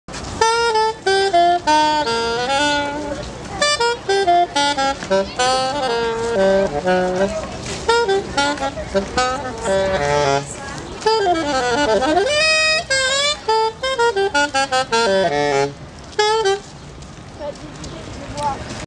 sax in the park 2
plaing the sax in central park